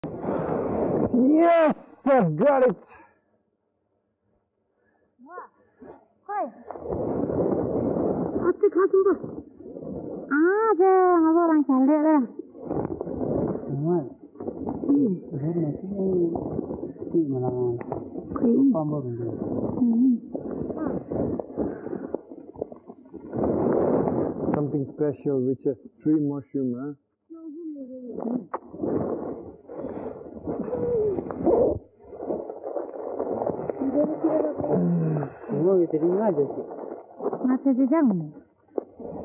Mingmar Lama - Tsum Valley, Nepal - Mingmar Finds More Yartsa Gunbu
“Yes, I got it!”

Mingmar Lama, Caterpillar Fungus Harvester, Tsum Valley, Nepal | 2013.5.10 • 2:37 PM | CC-BY-NC-SA 4.0